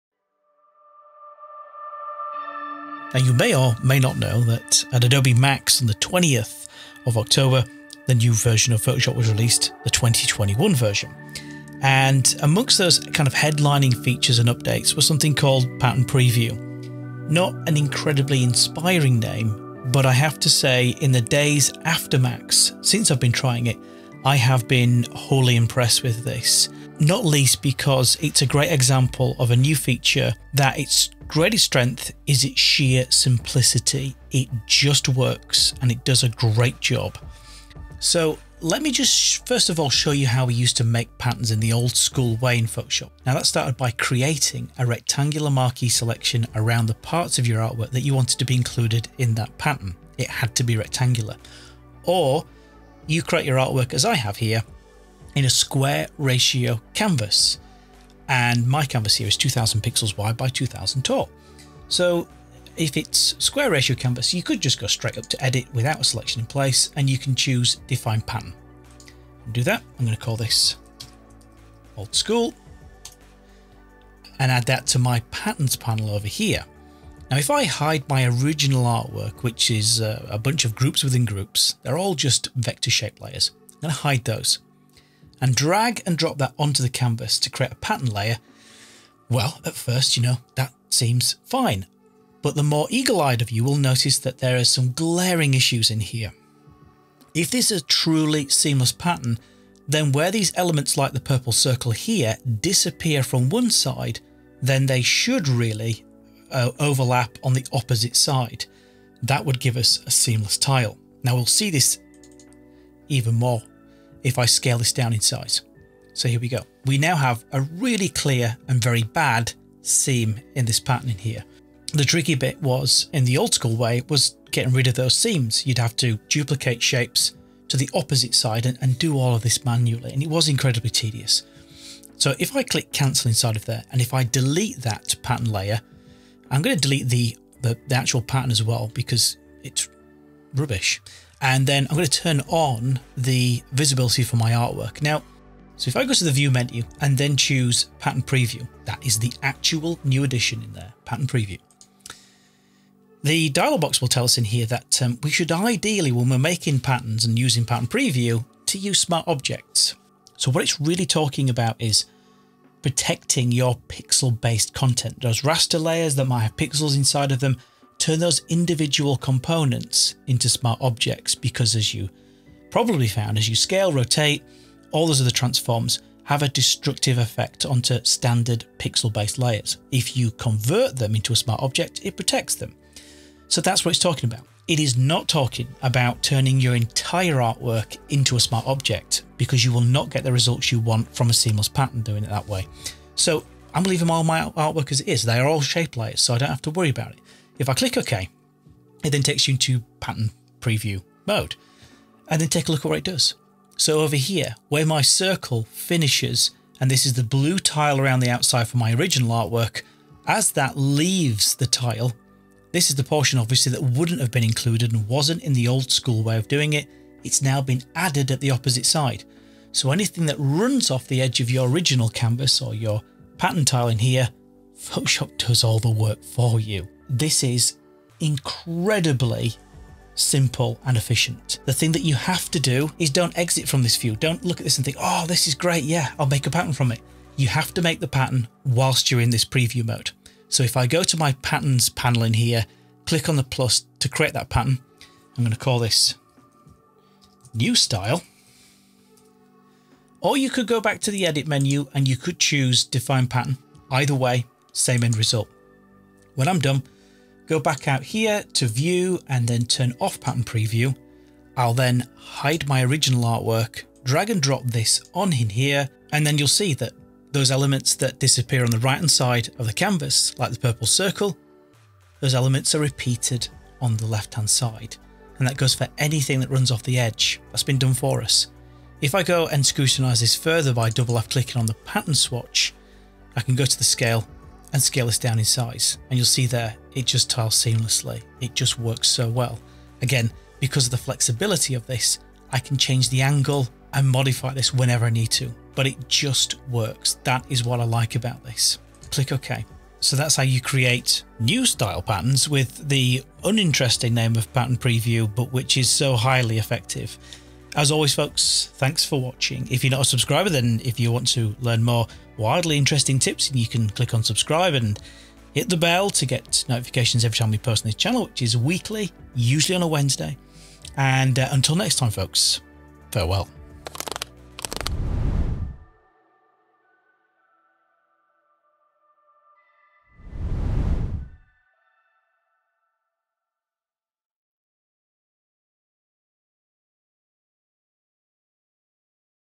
Photoshop 2021 New Feature | Pattern Preview
So with the arrival of the 2021 version of Adobe Photoshop at MAX came a handful of new features and updates. Pattern Preview didn't catch my eye for its uninspiring name, but having used it since, I have been wholly impressed with how elegantly simple it is to create seamless patterns, especially when using vector shape layers.

00:00 Introduction
00:38 The old way of making photoshop patterns
02:25 How to use pattern preview
03:50 Save your pattern
04:21 Test your new pattern as an adjustment layer